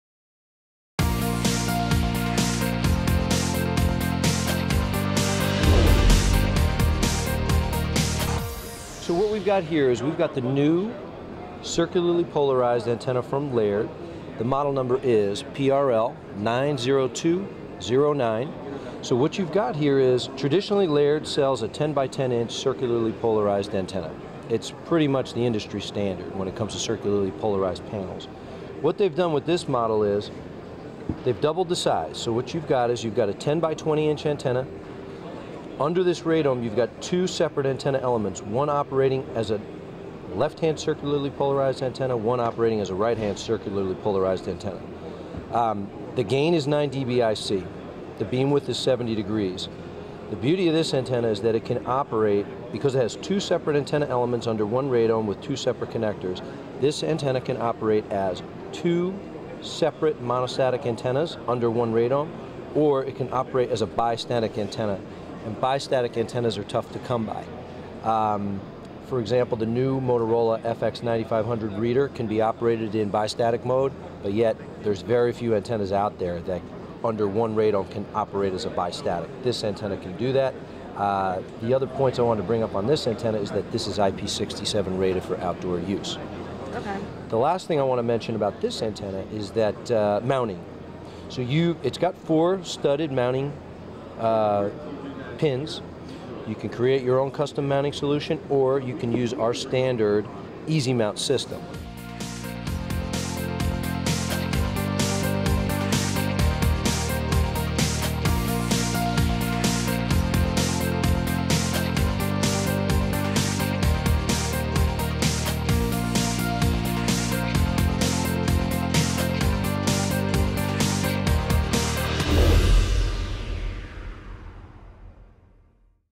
Laird Bistatic PRL90209 (CP) RFID Antenna (902-928 MHz)
We attended RFID Journal LIVE! 2014. While there, we spoke to Laird Technologies about their Bistatic PRL90209 (CP) RFID Antenna (902-928 MHz).